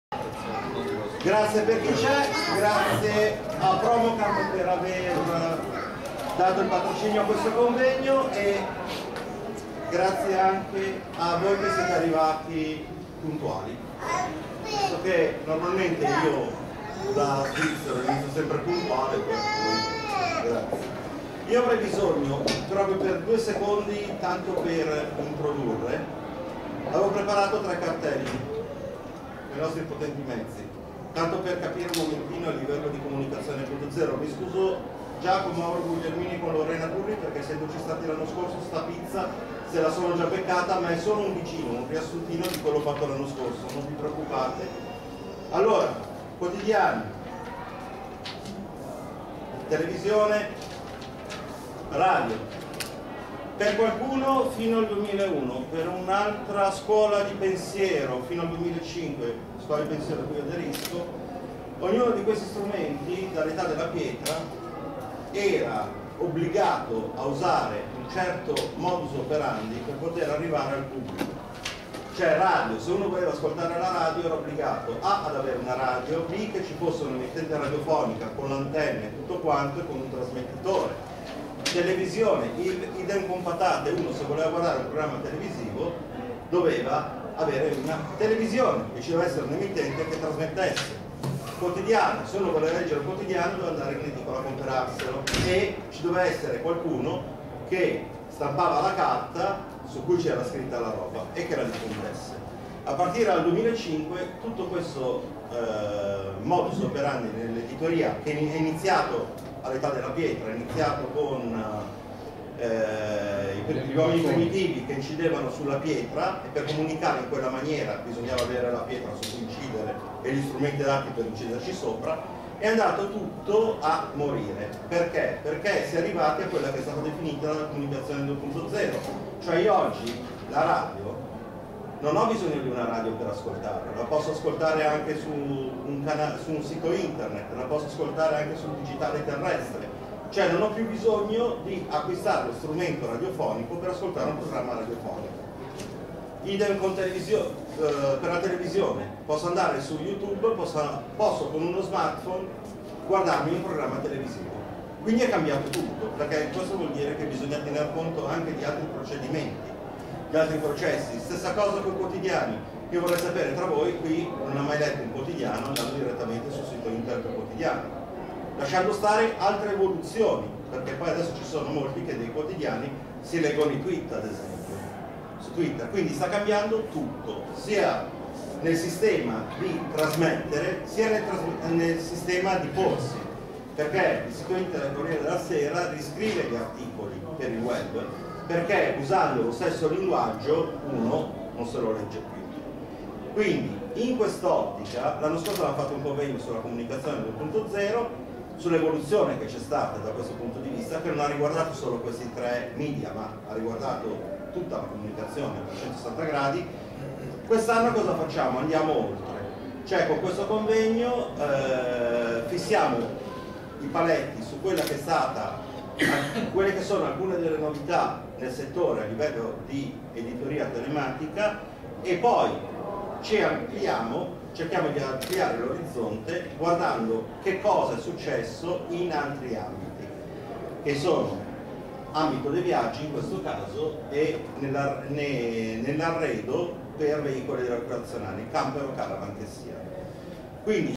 Tour.it 2012 - Comunicazione 2.0 Campus 2012 - Introduzione
Per i lettori di Newscamp presentiamo l'introduzione di Luca Stella al Convegno "Comunicazione 2.0 Campus 2011" che si è tenuto sabato, 14 gennaio, durante Tour.it, presso CarraraFiere